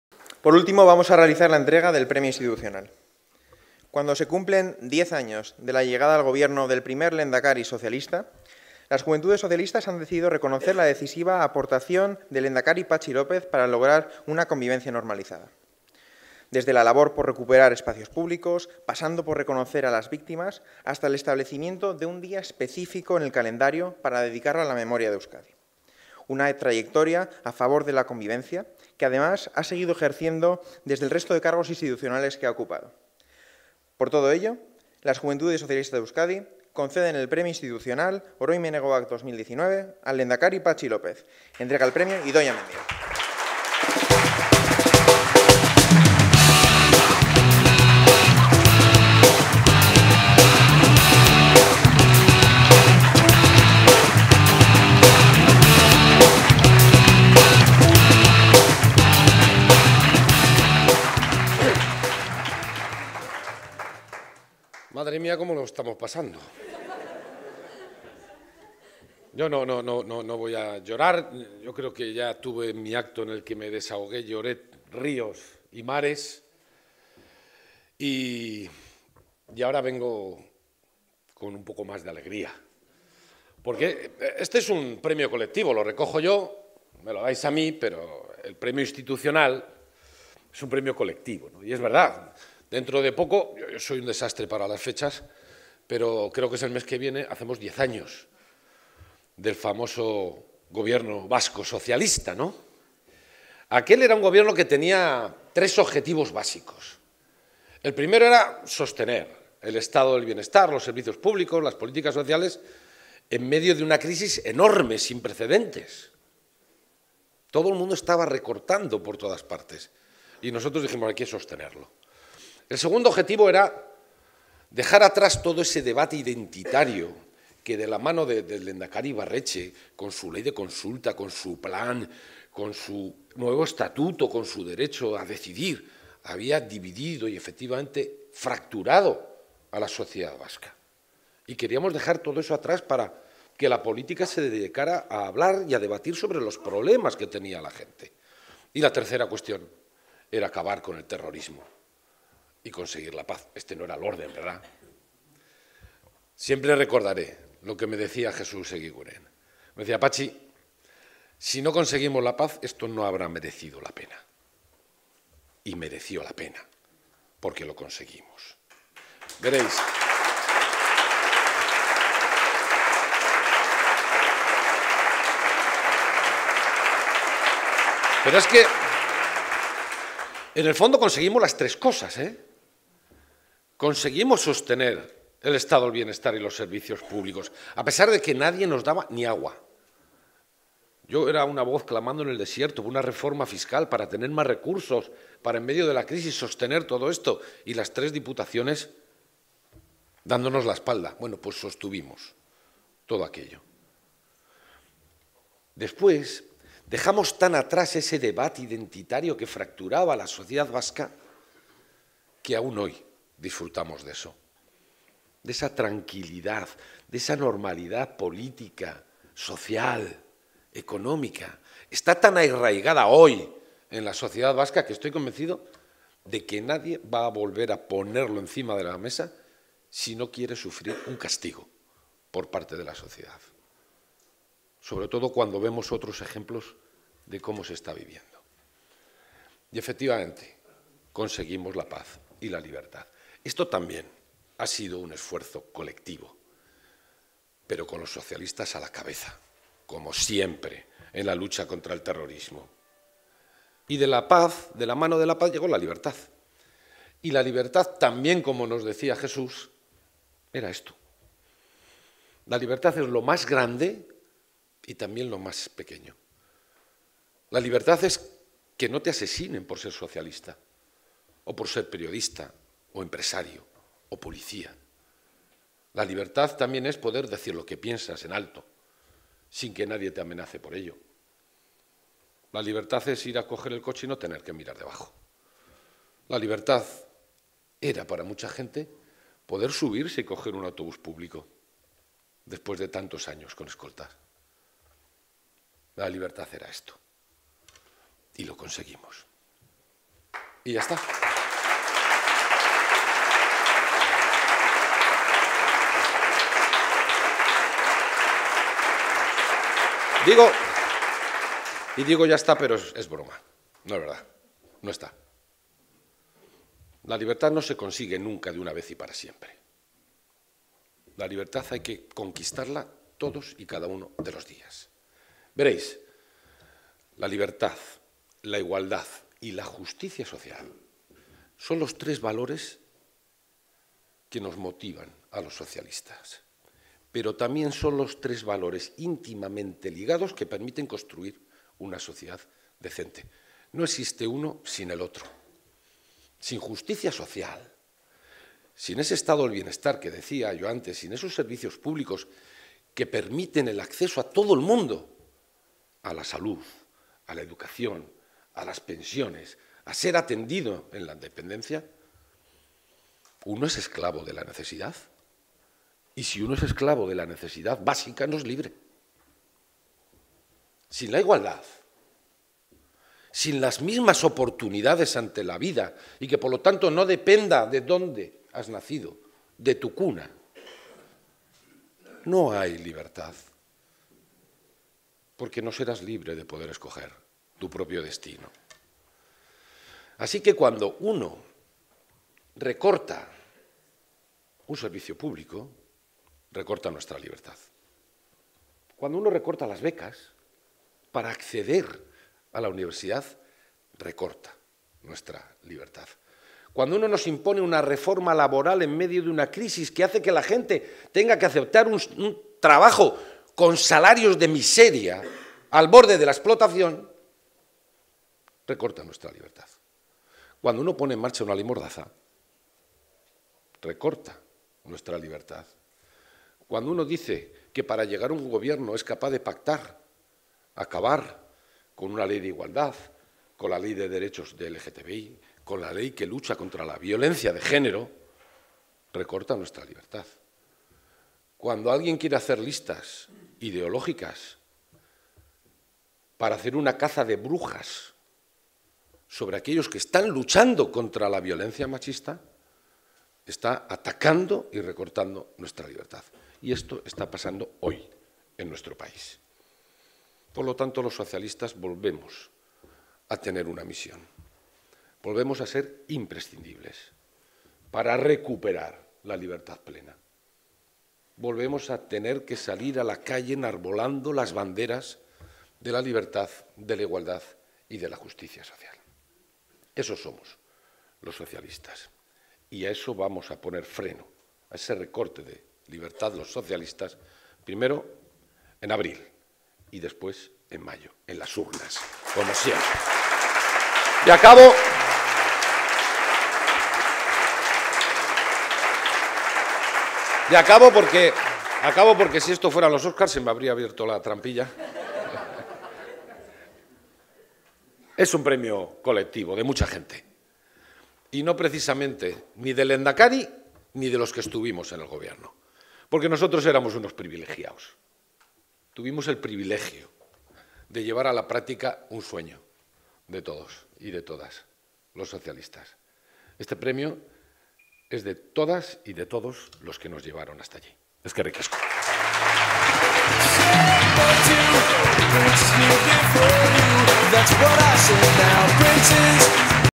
Premio Institucional Oroimen Hegoak 2019 a Patxi López
Gracias a las Juventudes Socialistas de Euskadi por este reconocimiento a la labor del Gobierno socialista de Euskadi que conquistó la libertad frente al terrorismo de ETA.